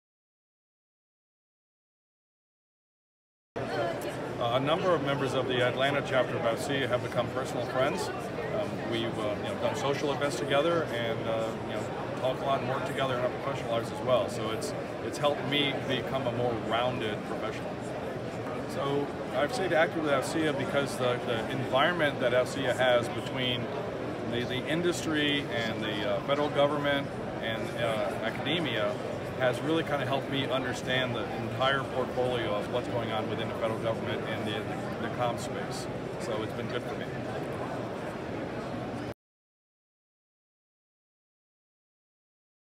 AFCEA In Your Words with Paul Wertz
In our testimonial video series "In Your Words," Paul Wertz, AFCEA member and CEO and President of Technology Sherpa, shares how the friendships he has developed from being a member have helped make him a more rounded professional.